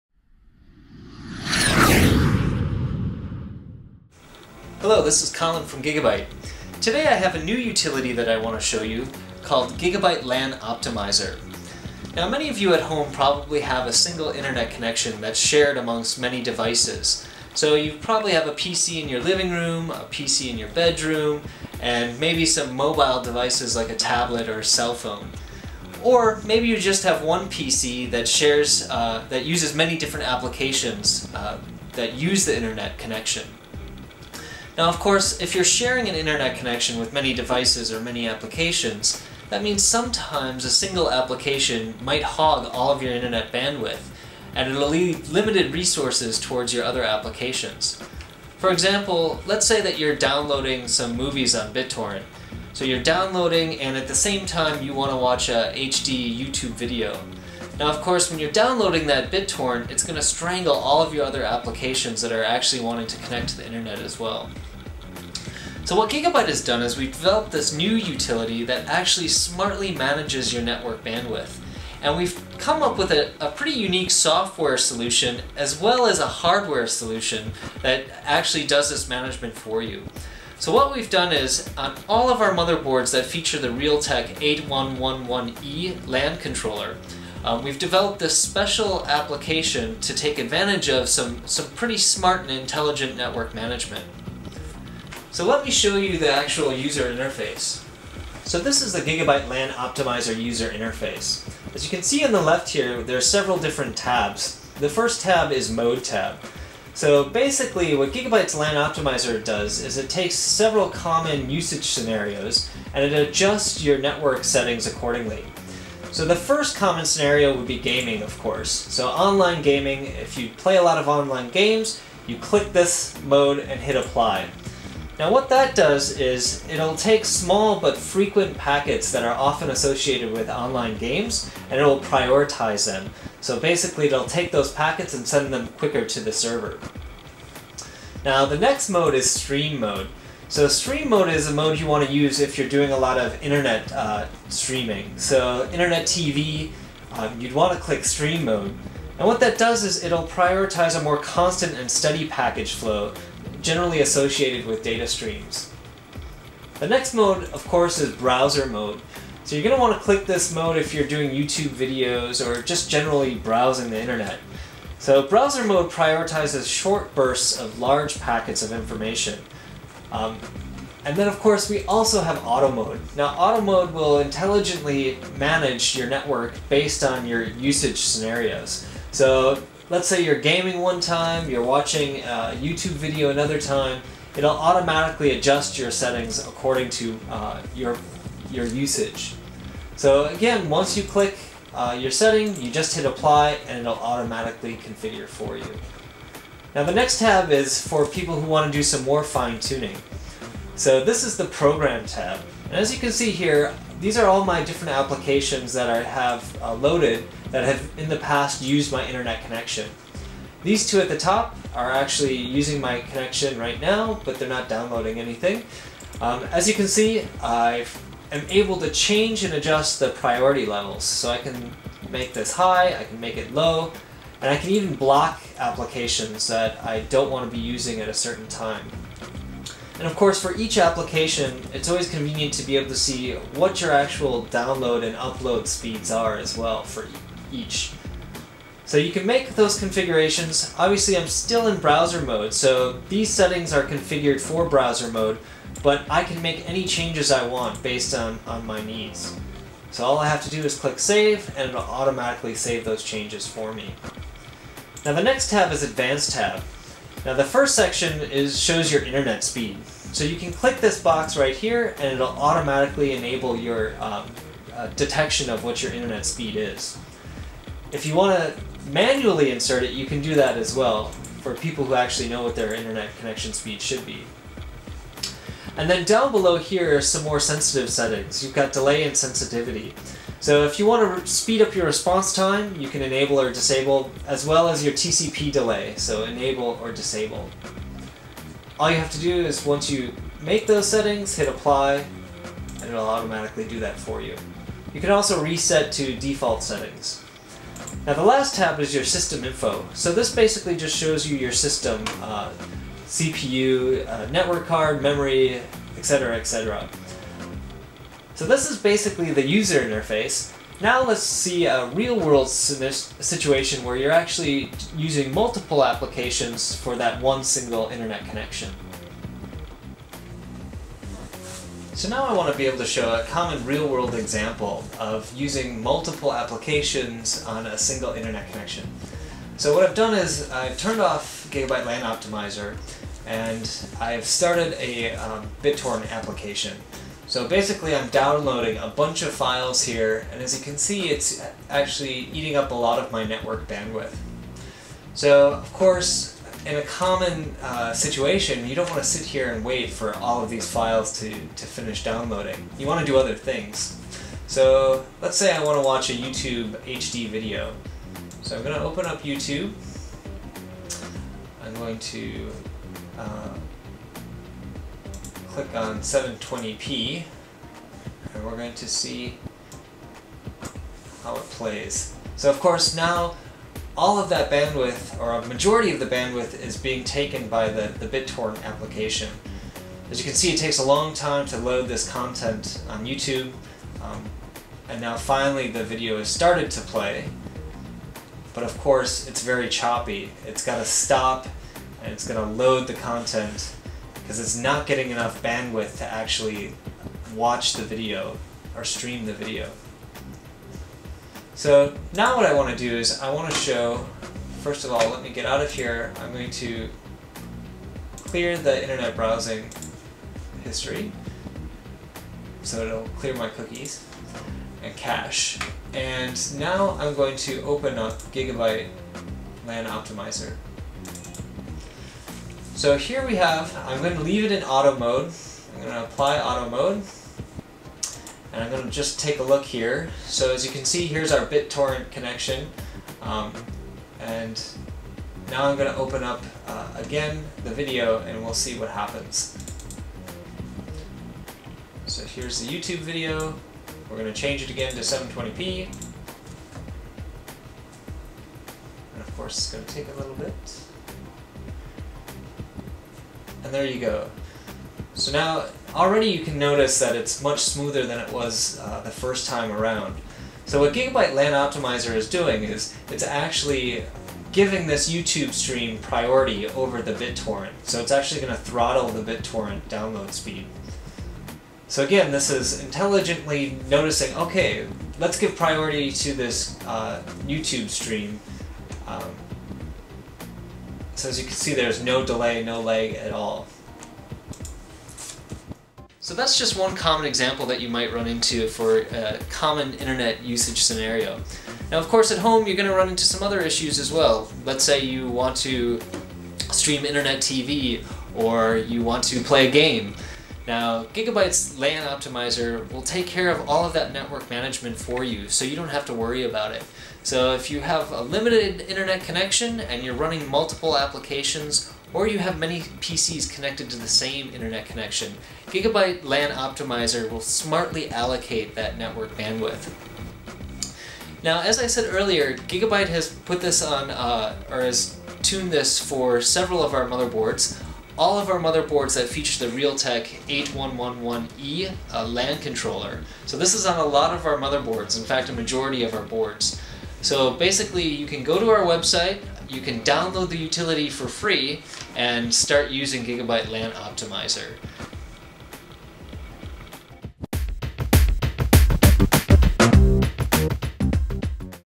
GIGABYTE Lan Optimizer
Intelligent Network Management, Made Easy.GIGABYTE LAN Optimizer is a simple GUI-based, software application that works in conjunction with the Realtek 8111E and above Network Interface Controller (NIC). It's been designed to be as user friendly as possible, making intelligent network management simple to understand and implement.